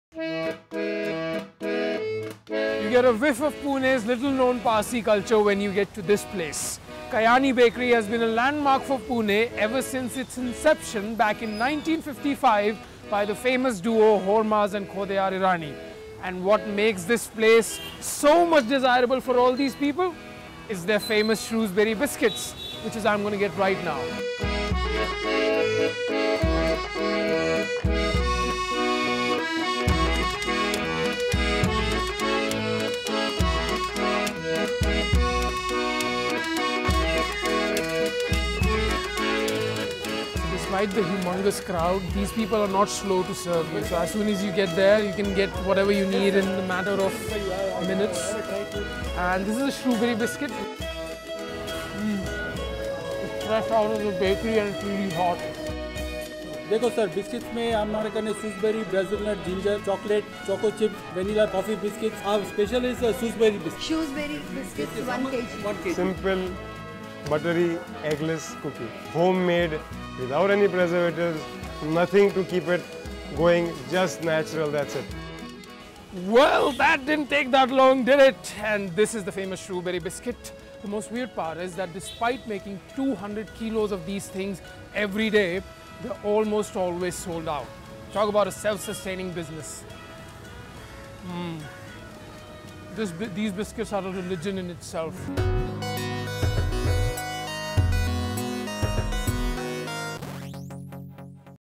Famous Kayani Bakery of Pune
Kayani Bakery is a British Raj-era Cafe that seems to be stuck in a time wrap. Famous for its Shrewsbury biscuits, madeira and sponge cakes. Do visit